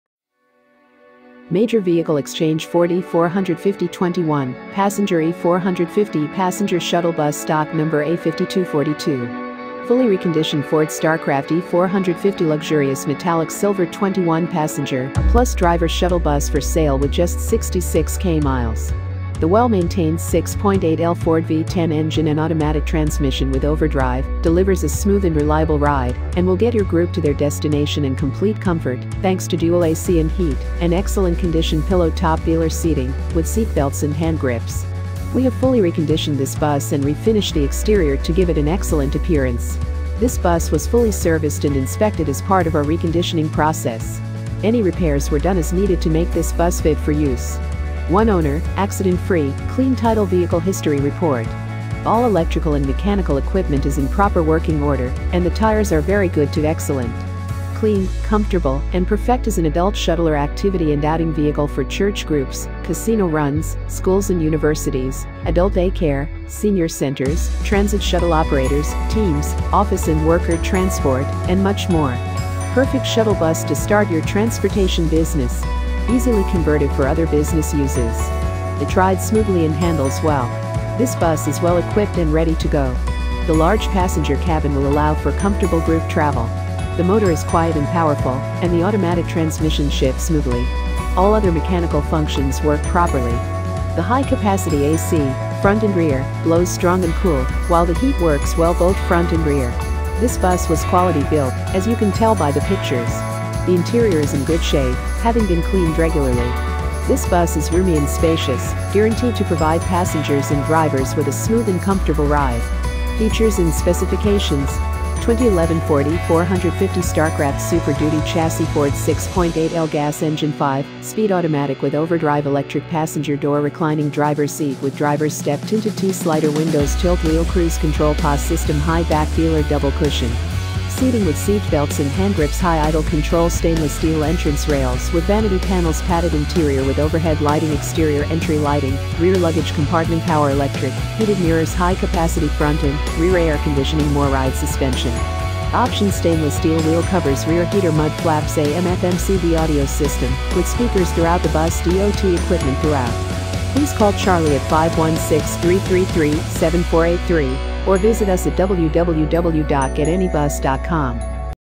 2011 Luxury Ford E450 21 Passenger E450 Passenger Shuttle Bus w/ only 66K Miles!
Fully Reconditioned Ford Starcraft E-450 luxurious metallic silver 21-passenger plus driver shuttle bus for sale with just 66k miles. The well-maintained 6.8L Ford V-10 engine and automatic transmission with overdrive delivers a smooth and reliable ride and will get your group to their destination in complete comfort thanks to dual A/C and heat and excellent condition pillow top velour seating w/ seatbelts and handgrips. We have fully reconditioned this bus and refinished the exterior to give it an excellent appearance. This bus was fully serviced and inspected as part of our reconditioning process. Any repairs were done as needed to make this bus fit for use. One owner, accident-free, clean title vehicle history report.

All electrical and mechanical equipment is in proper working order, and the tires are very good to excellent. Clean, comfortable, and perfect as an adult shuttle or activity and outing vehicle for church groups, casino runs, schools & universities, adult day care, senior centers, transit shuttle operators, teams, office and worker transport, and much more! Perfect shuttle bus to start your transportation business! Easily converted for other business uses.

It rides smoothly and handles well. This bus is well-equipped and ready to go. The large passenger cabin will allow for comfortable group travel. The motor is quiet and powerful, and the automatic transmission shifts smoothly.  All other mechanical functions work properly. The high capacity A/C (front and rear) blows strong and cool, while the heat works well both front and rear. This bus was quality built, as you can tell by the pictures. The interior is in good shape, having been cleaned regularly. This bus is roomy and spacious, guaranteed to provide passengers and drivers with a smooth and comfortable ride.

FEATURES & SPECIFICATIONS: 2011 Ford E-450 Starcraft Super Duty Chassis - Ford 6.8L Gas Engine - 5 Speed Automatic With Overdrive - Electric Passenger Door - Reclining Drivers Seat w/ Drivers Step - Tinted T-Slider Windows - Tilt Wheel - Cruise Control - PA System - High Back Velour Double Cushion Seating w/ Seatbelts and Handgrips - High Idle Control - Stainless Steel Entrance Rails with Vanity Panels - Padded Interior w/ Overhead Lighting - Exterior Entry Lighting - Rear Luggage Compartment  - Power Electric Heated Mirrors - High Capacity Front & Rear Air Conditioning - Mor/Ryde Suspension Option - Stainless Steel Wheel Covers - Rear Heater - Mud Flaps - AM/FM/CD Audio System with Speakers Throughout the bus- DOT Equipment Throughout.